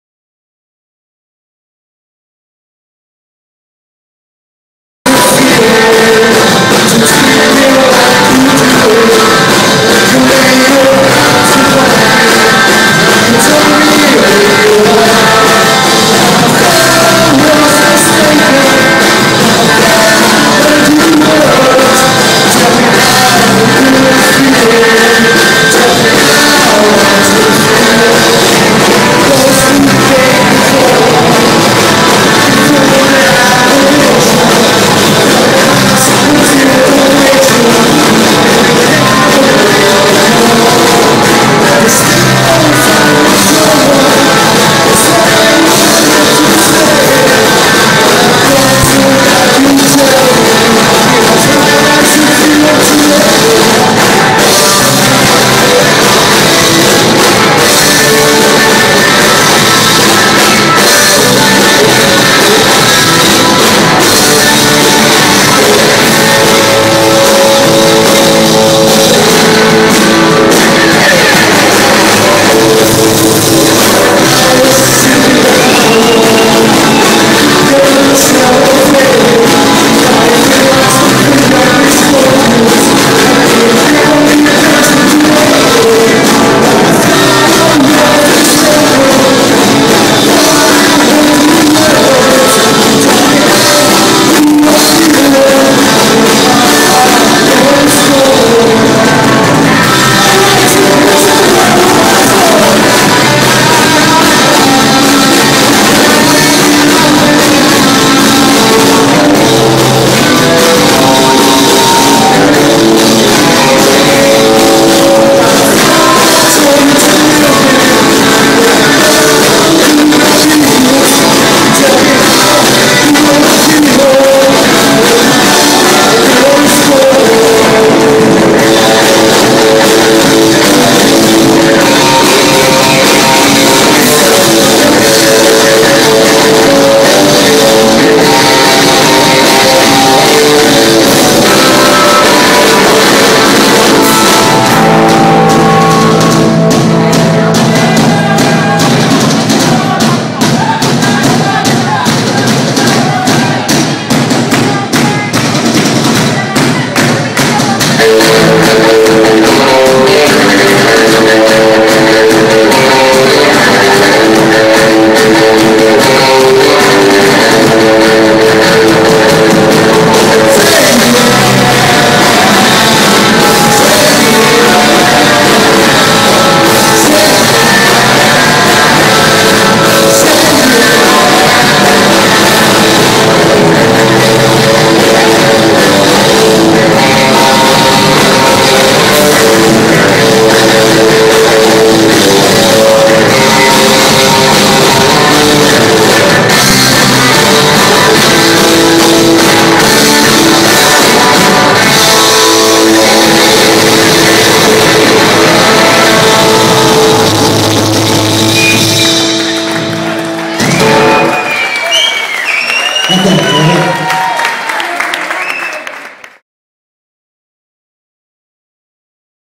avantguarda.com / Dr. Explosión @ Stay Sick - KGB Barcelona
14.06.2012 - Concert inaugural del cicle de garatge Stay Sick al KGB de Barcelona, amb la presència de Doctor Explosión i els barcelonins Fuckin Bollocks.